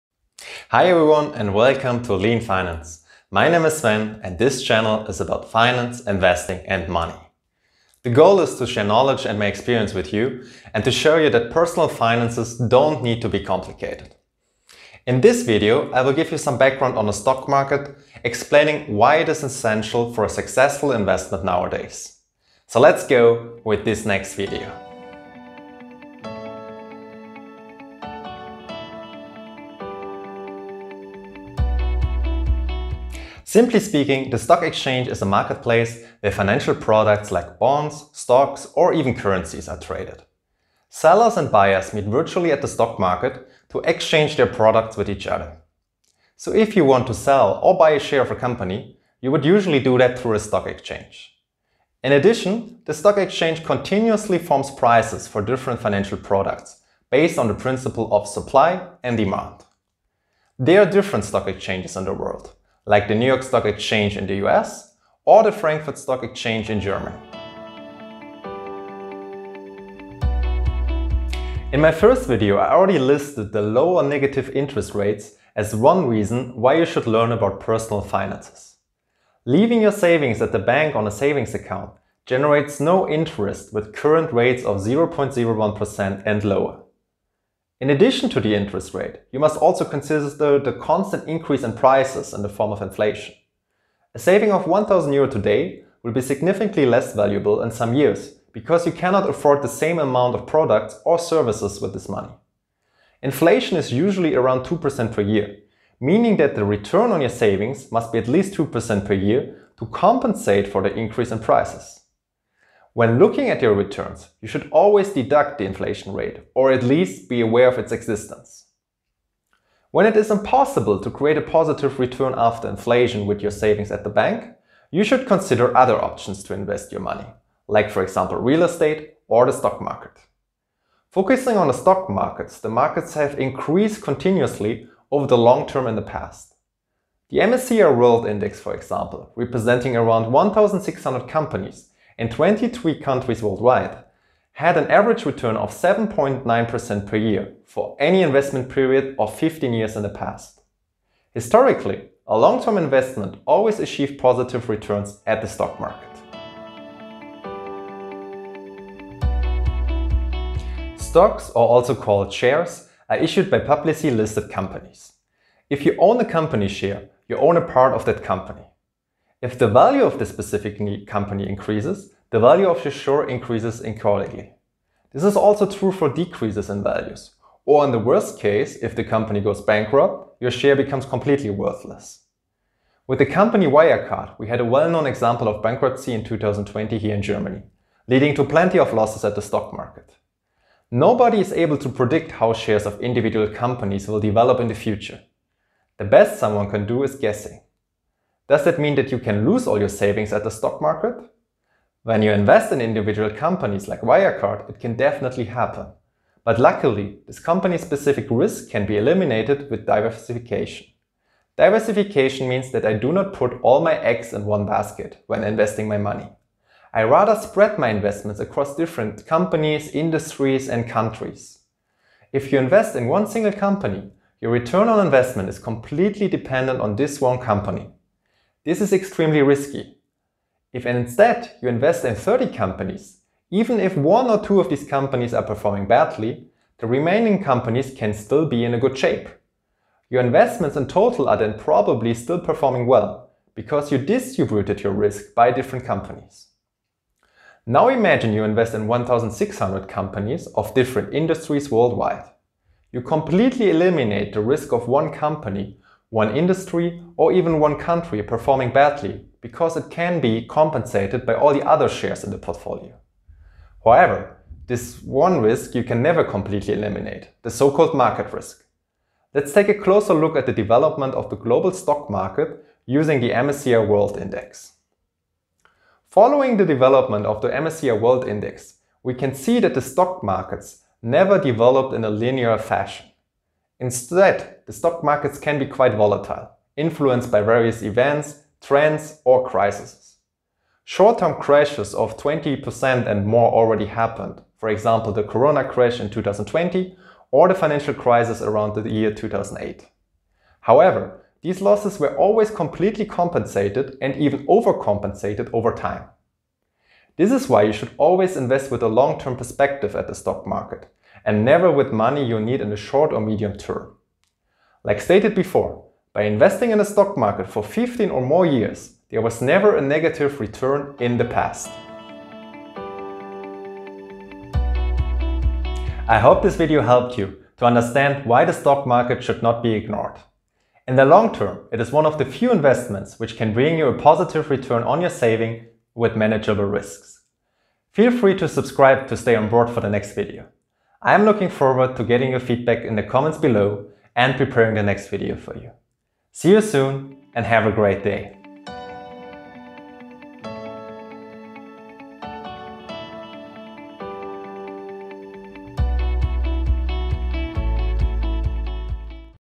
Why you cannot IGNORE the STOCK MARKET
Hello everyone and welcome to this video! In this video I will give you some background on the stock market, explaining why it is essential for a successful investment nowadays and how to manage the risks.

I am happy, when you like the produced content and it inspires you to start taking care of your own personal finances.